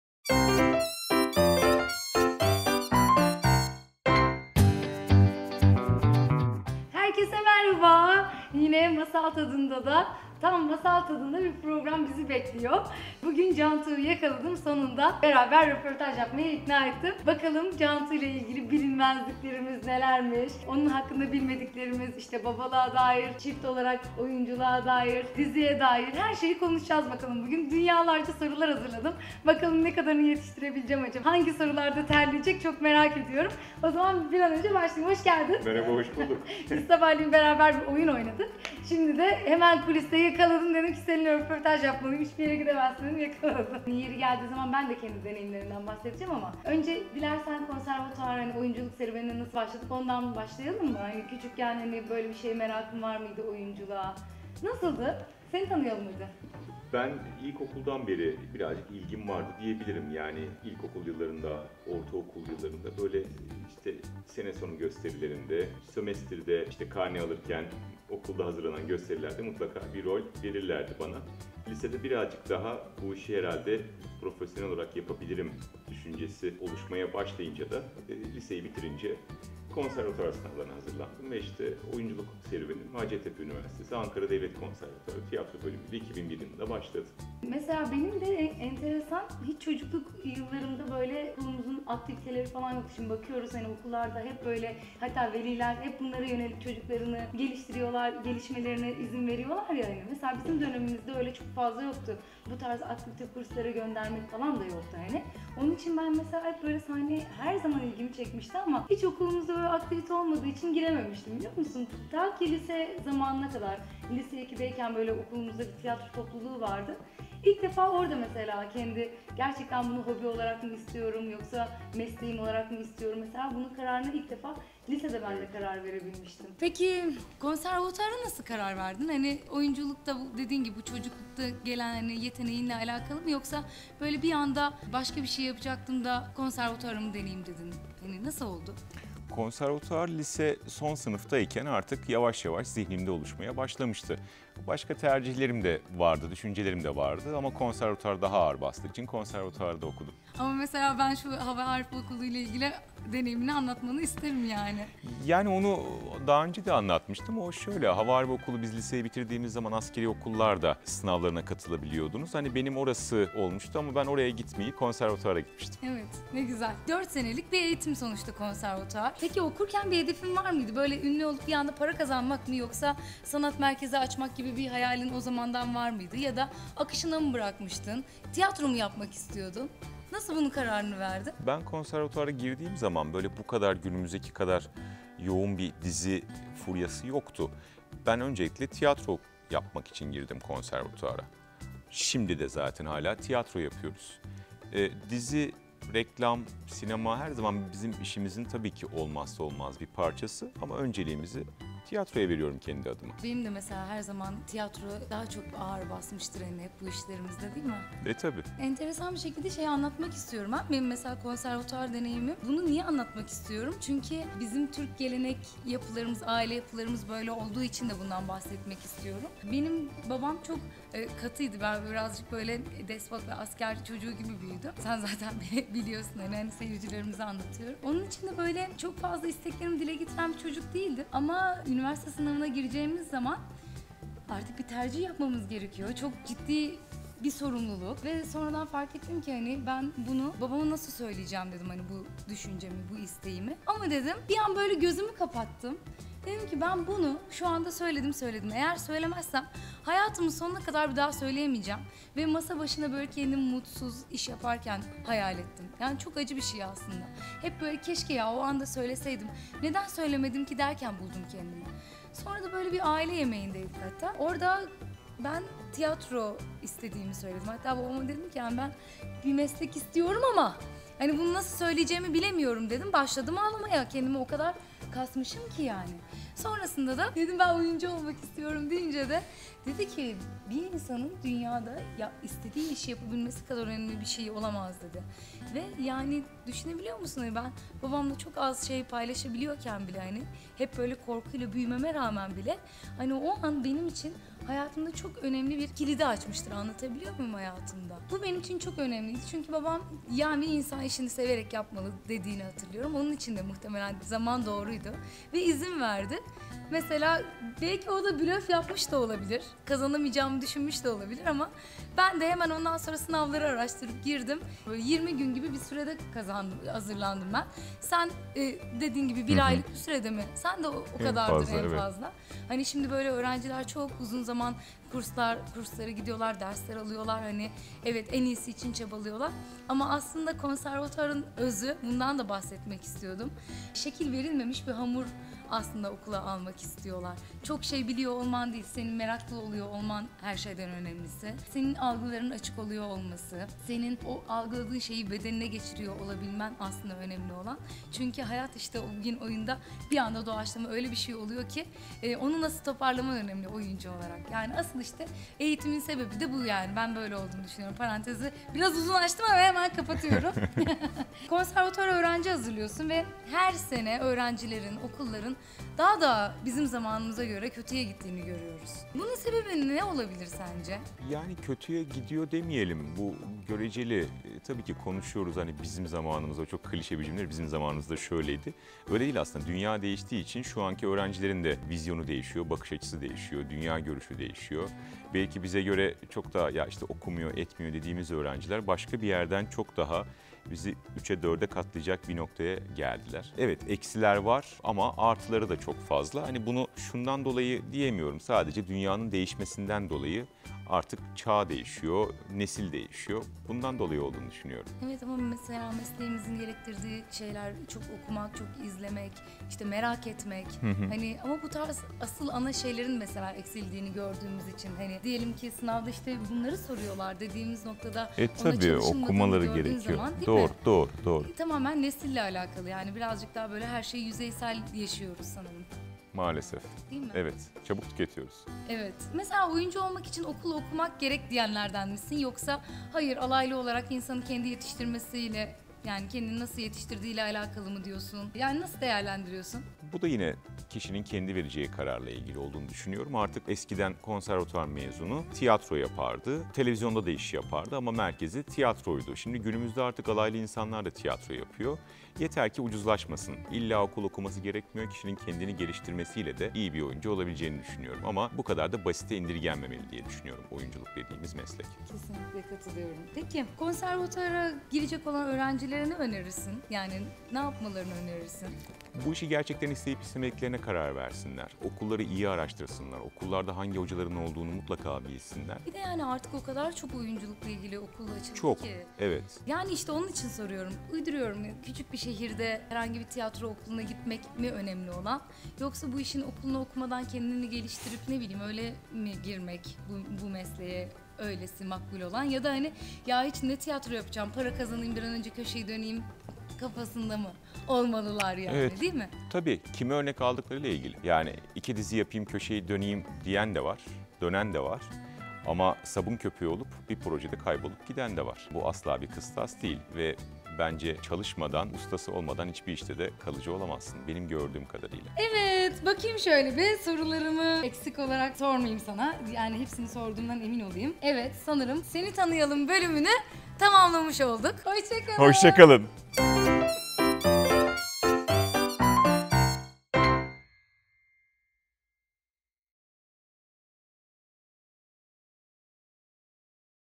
CANTUĞ TURAY İLE MASAL TADINDA BİR SOHBET | Begüm Topçu Turay ile Masal Tadında
Cantuğ ile masal tadında bir sohbet. Cantuğ Turay hakkında bilmediklerimize dair, oyunculuk serüvenine dair, babalığa dair her şeye dair çok keyifli ve sıcak bir sohbet yaptık.

Merak edenler haydi videoya!

Haydi, SENDE YAPABİLİRSİN!

►Her Hafta Yeni, Video Yayında!

© Tüm Hakları Monitör Ajans'a Aittir.